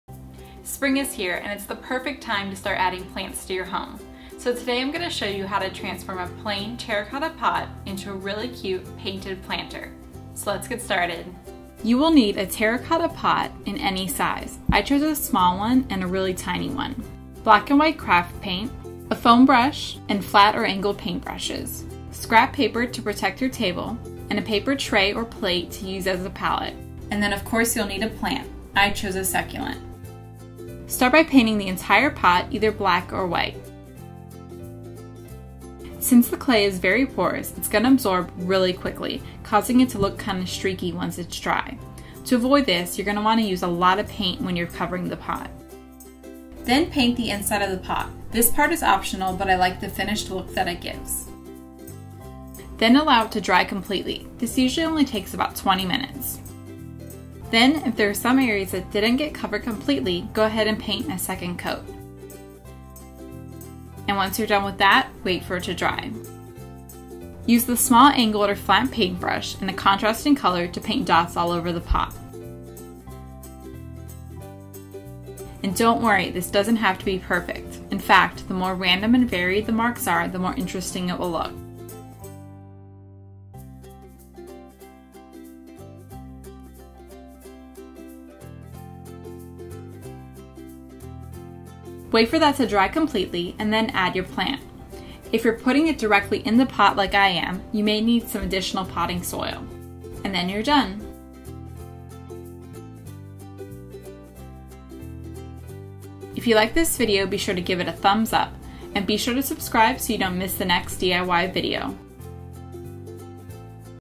Painted Spotted Dot Terra Cotta Clay Pots
Learn how easy it is to add some green to your home with these fun spotted painted terra cotta pots!

SUPPLIES:
- terra cotta pot
- black and white craft paint
- foam brush
- small flat or angeles paint brush, the larger the brush the larger the spots will be
- scrap paper
- paper tray or plate for your paint
- plant
- optional: potting soil